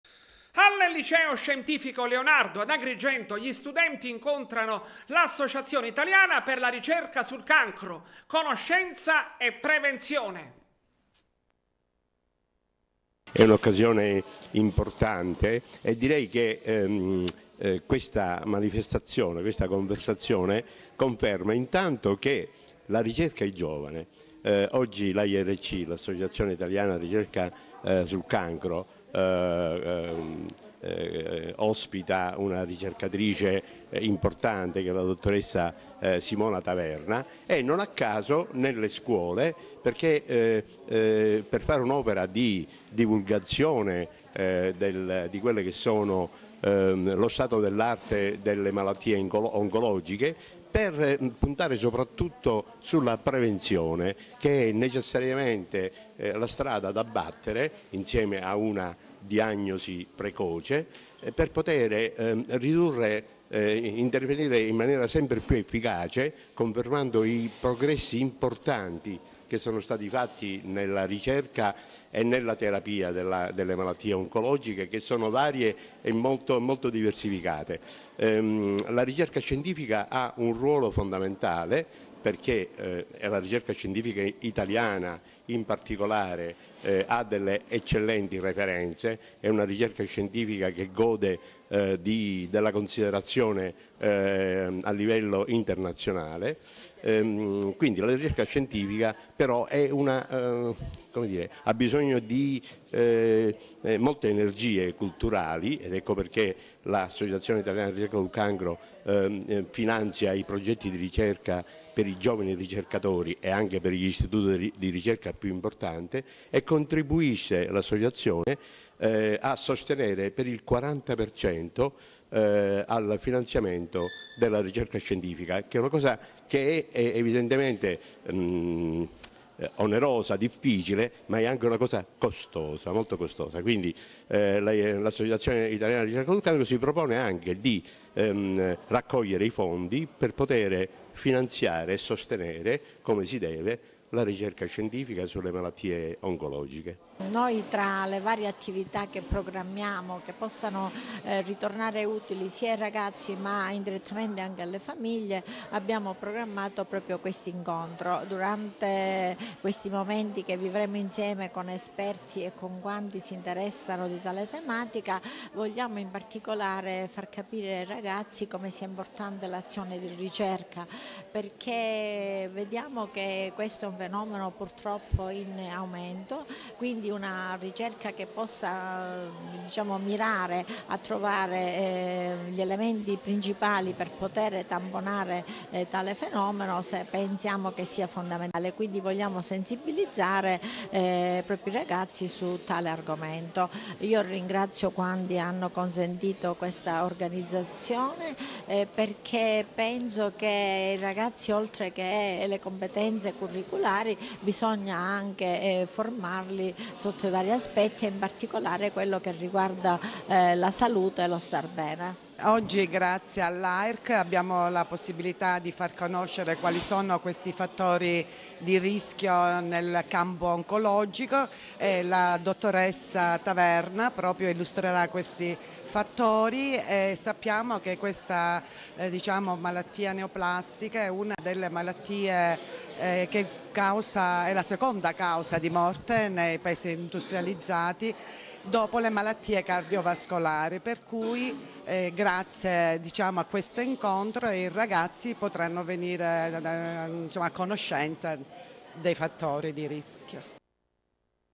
Teleacras - Scuole e prevenzione oncologica
Al Liceo scientifico Leonardo ad Agrigento gli studenti incontrano l’Associazione italiana per la ricerca sul cancro. Conoscenza e prevenzione.